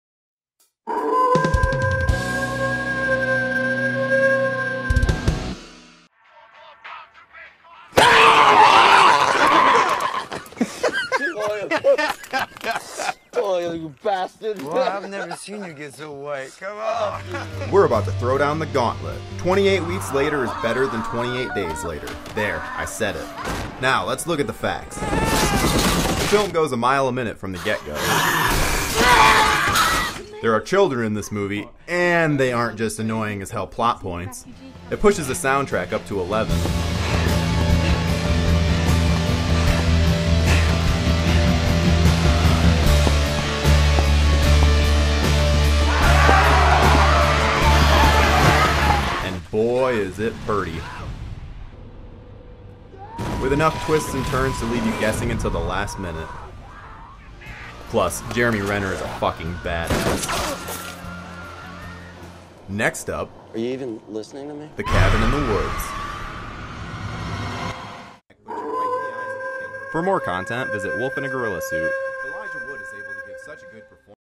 31 Days of Halloween - Day 22 "28 Weeks Later"
Halloween is the best. So in order to properly celebrate we thought we'd spotlight 31 different horror movies every day throughout the month of October!

Check back tomorrow for more!